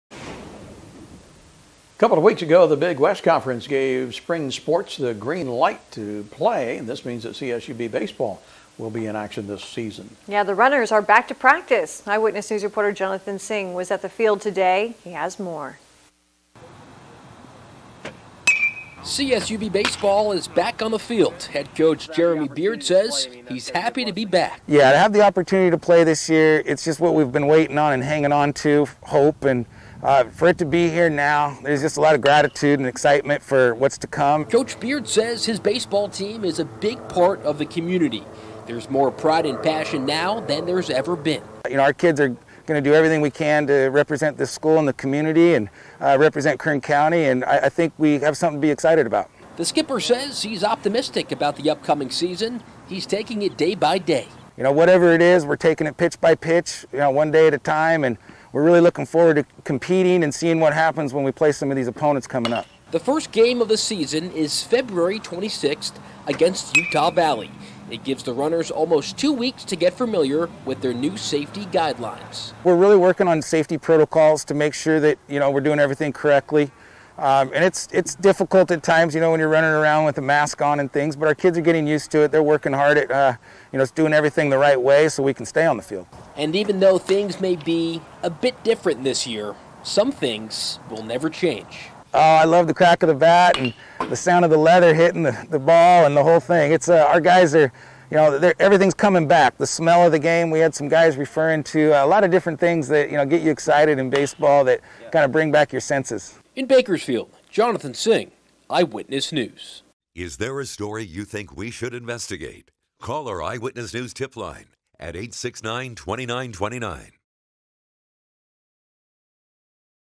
CSUB Baseball Back On The Field
Cal State Bakersfield Baseball returns to practice. The Runners enter their first season in the Big West Conference.

Their first game is February 26th against Utah Valley. It will be the start of twelve straight home games as CSUB opens up their season.